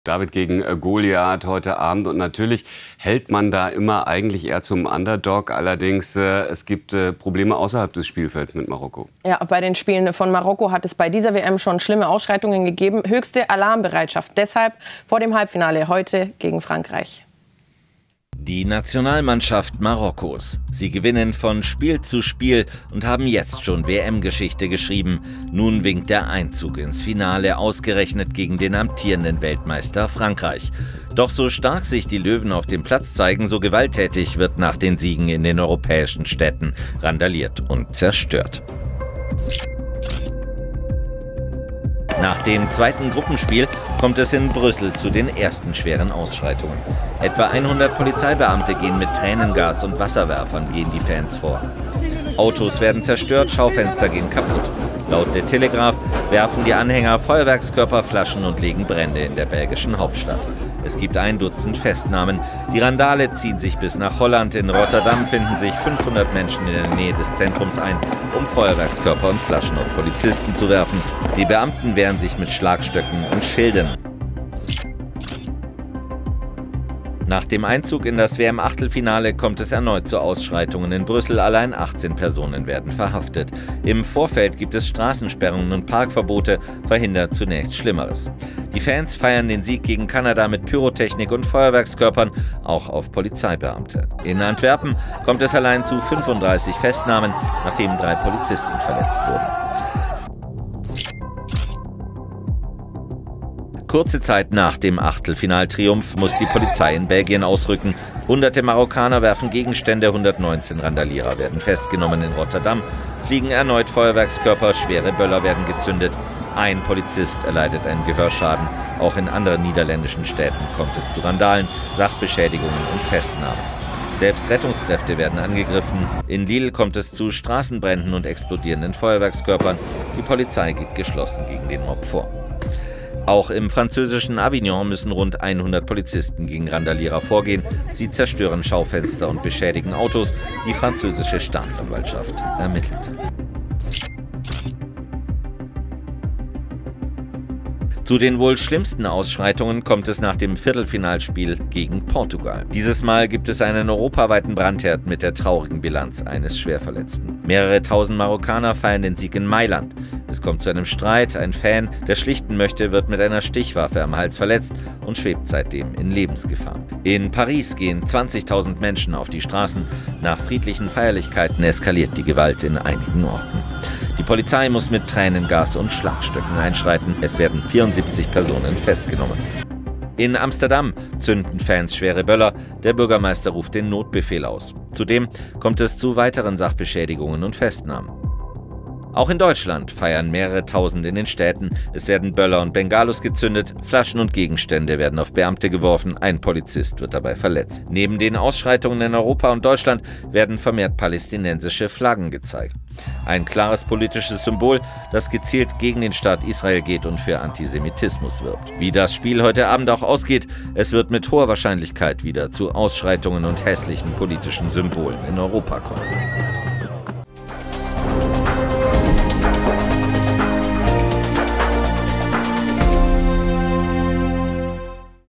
Vor Spiel gegen Marokko: Frankreich mobilisiert 10 000 Polizisten | WM-Halbfinale
Randale befürchtet! Frankreich mobilisiert für das WM-Halbfinale gegen Marokko am Mittwoch (20 Uhr/ZDF und MagentaTV) 10 000 Polizeibeamte. Die Hälfte davon soll im Großraum Paris eingesetzt werden. Das kündigte Innenminister Gérald Darmanin (40) am Dienstag an.

Hintergrund: Zuletzt kam es nach den Marokko-Siegen beim Wüsten-Turnier immer wieder zu heftigen Ausschreitungen!